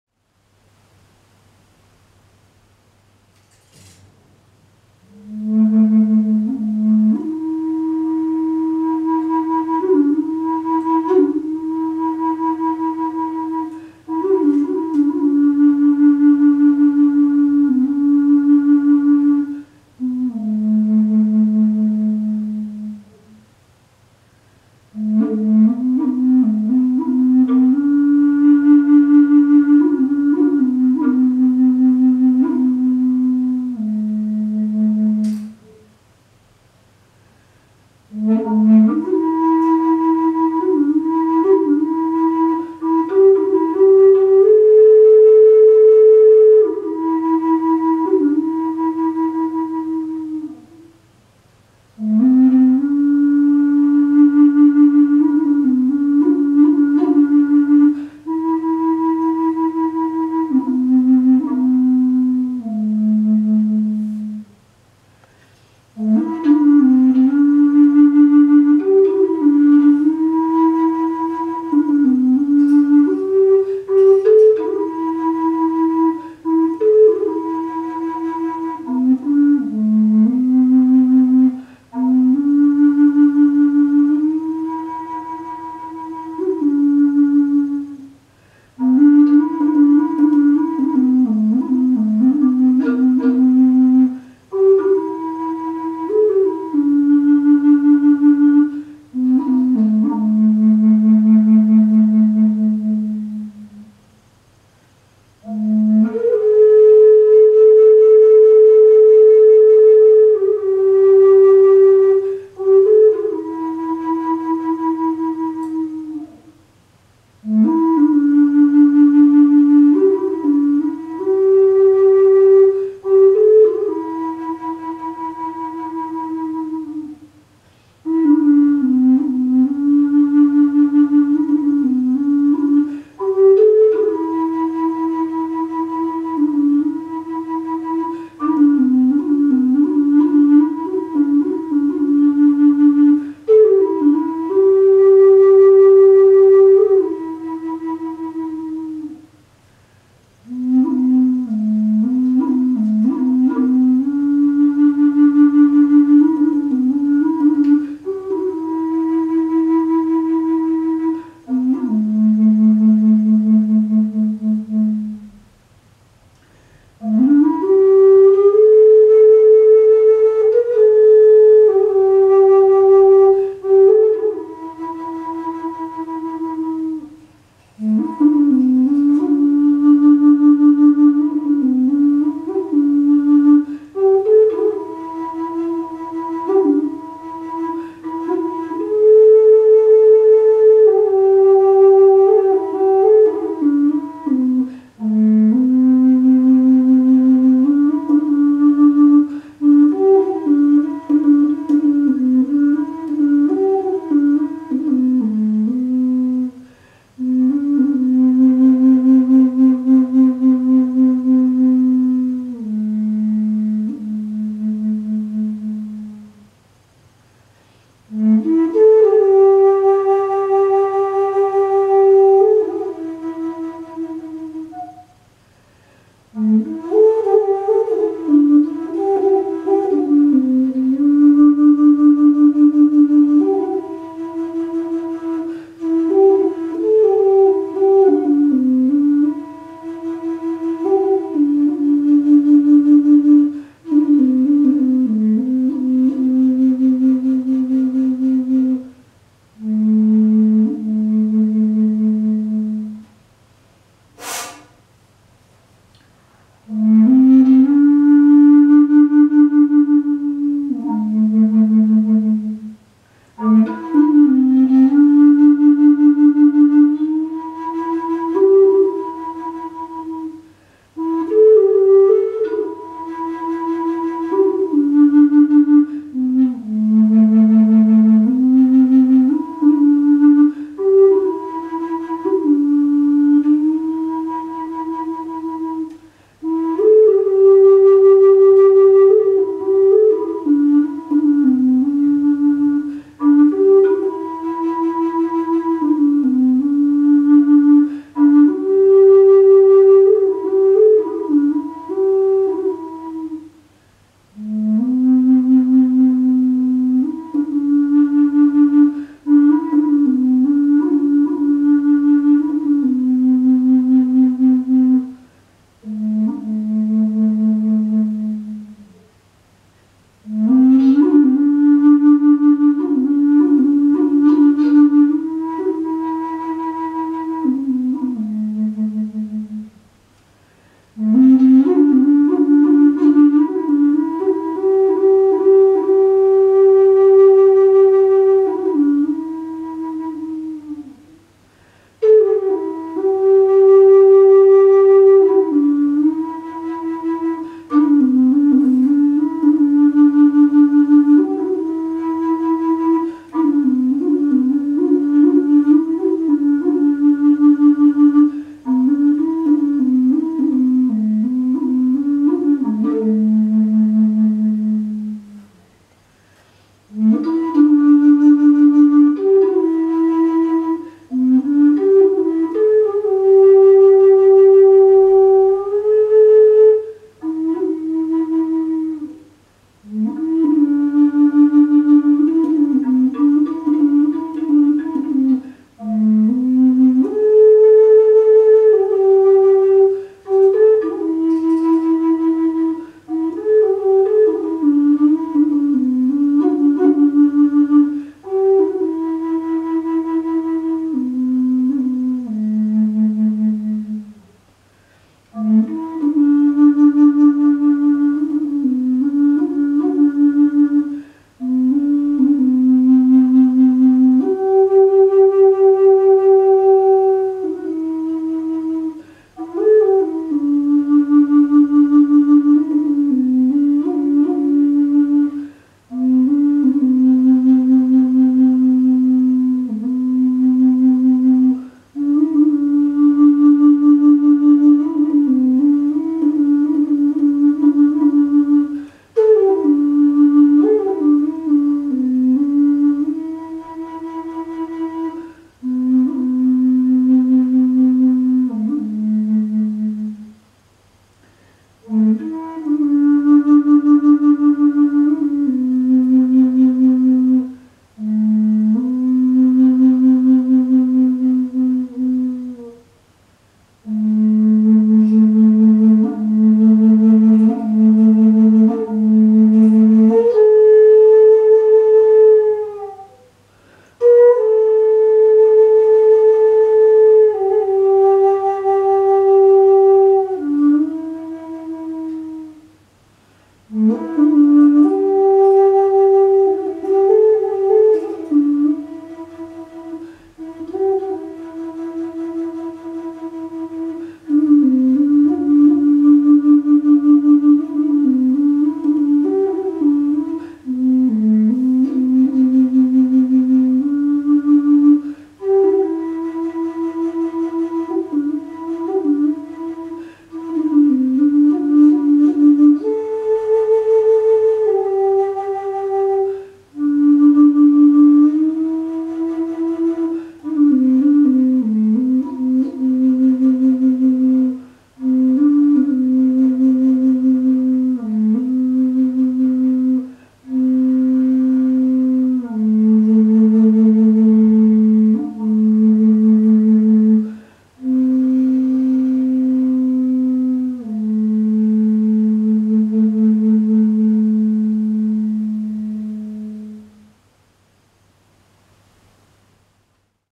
Rainy Day ♫ - High Spirits Contra Bass A
Indoor fluting on a rainy day.